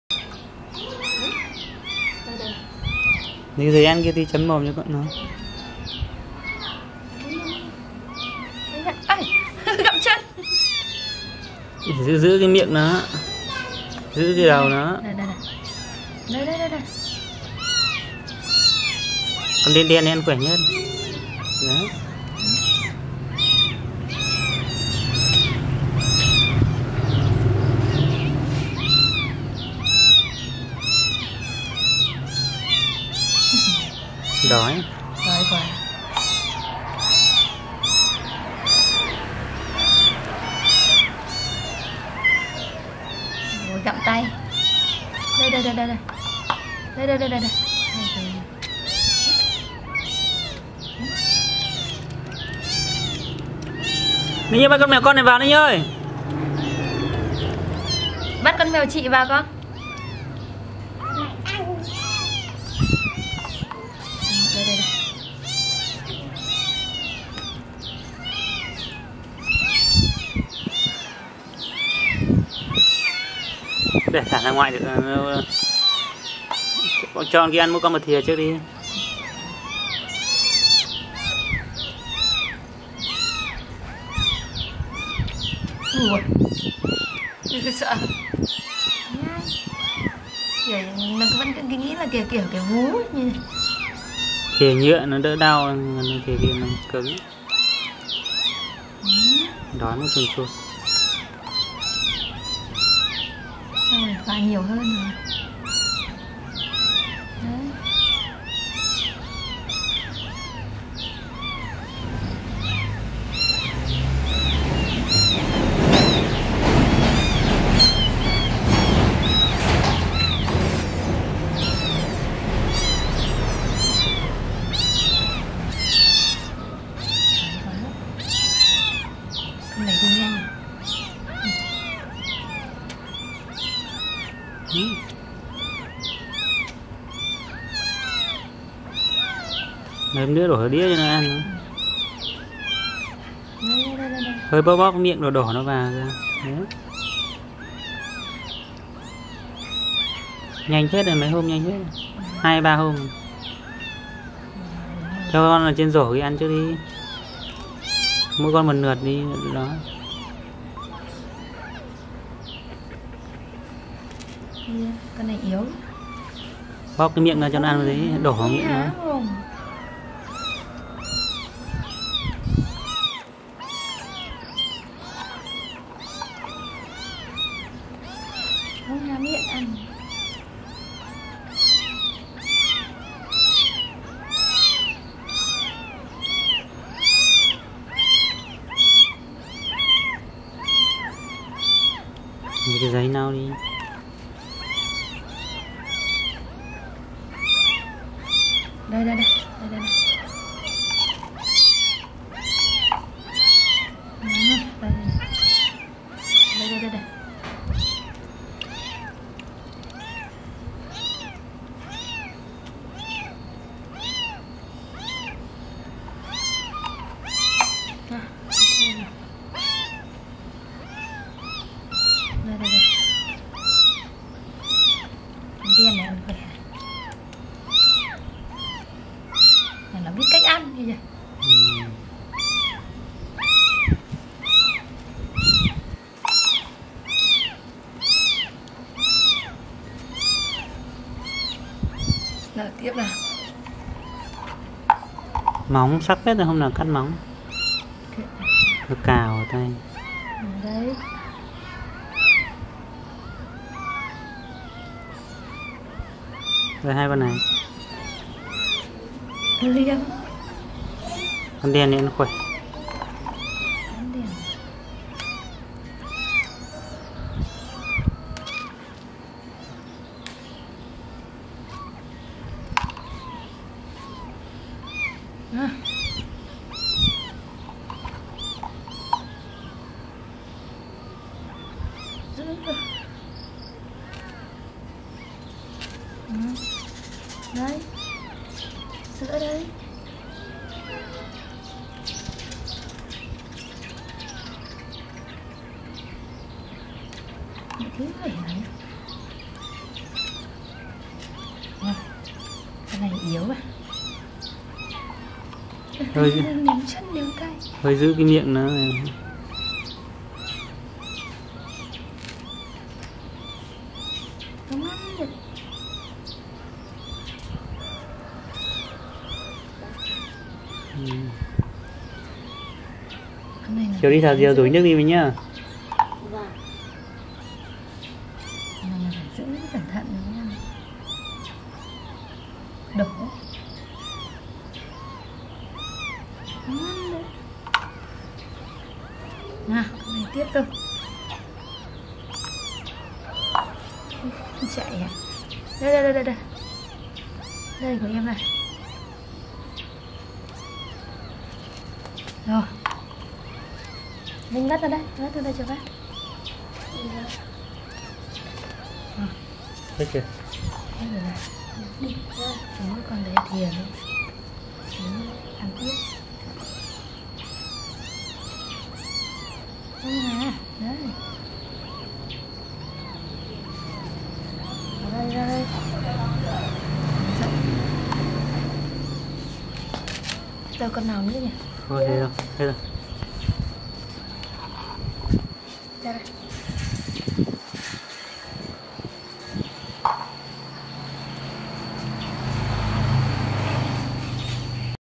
MÈO CON ĐÁNG THƯƠNG
Mèo mẹ bị tai nạn khi chúng còn nhỏ síu,mình khi vọng chăm chúng khoẻ mạnh